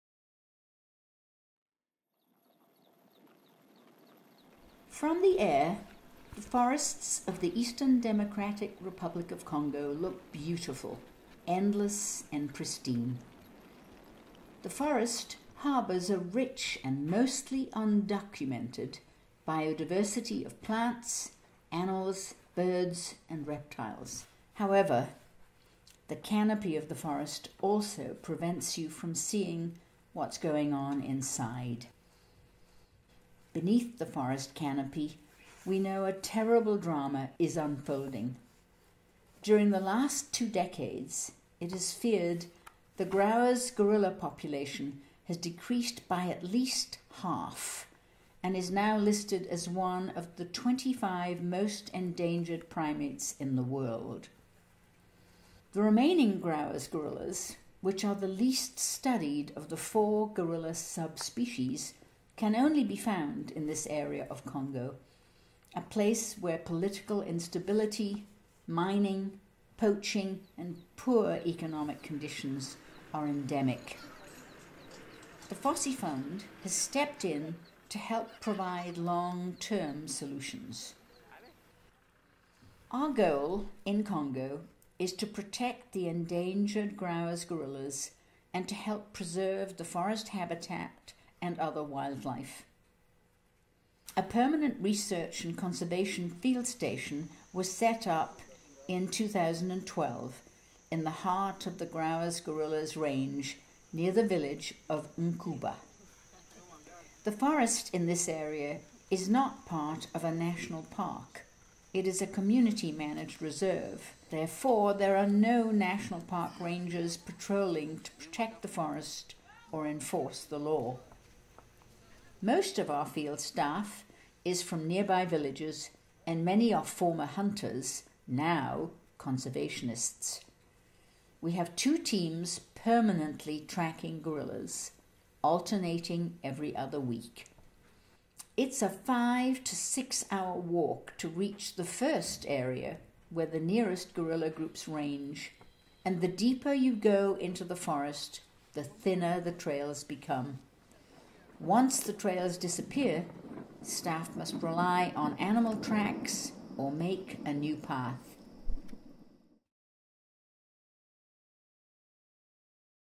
The Fossey Fund's work with Grauer's gorillas in Nkuba-Biruwe (Part 1)
Part 1: The Dian Fossey Gorilla Fund has stepped in to help provide long-term solutions for the Grauer’s gorilla in Democratic Republic of Congo. In 2012, our organization set up a permanent field site near the village of Nkuba to survey the surrounding forest for gorillas and other crucial biodiversity. During the last two decades, the Grauer’s gorilla population has decreased by at least half, and is now listed as one of the 25 most endangered primates in the world.